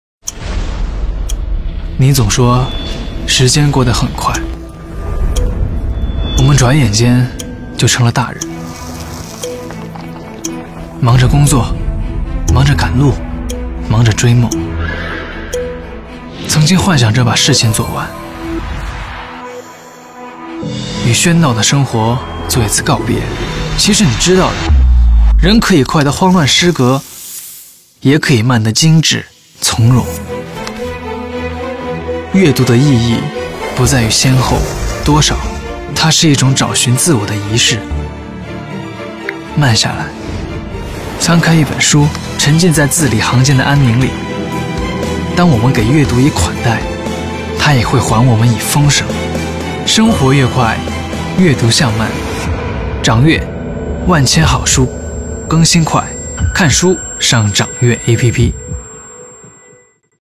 [Vương Tuấn Khải x iReader] Với phong cách nghệ thuật hậu hiện đại nói lên thái độ đọc sách
[Vương Tuấn Khải x iReader] Dùng clip với phong cách nghệ thuật hậu hiện đại thể hiện thái độ đọc sách của bản thân.